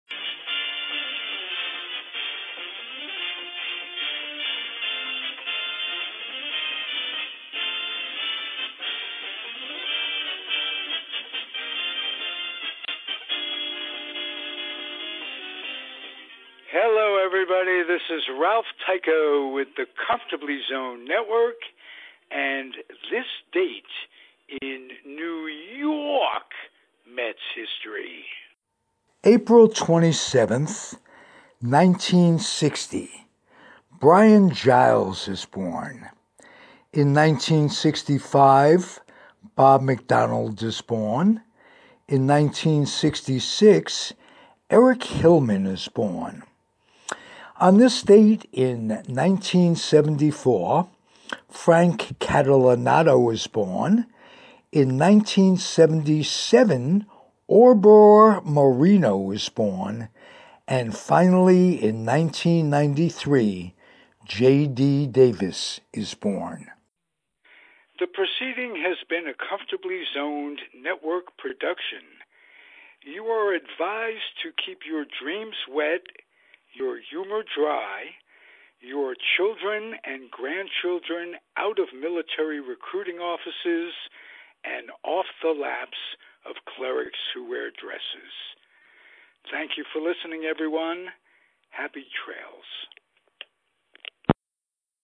This Date in New Yawk Mets' History 4/27
1960- Brian Giles is born.
1965- Bob MacDonald is born.
1966- Eric Hillman is born.
1974- Frank Catalanotto is born.
1977- Orber Moreno is born.
1993- J.D. Davis is born.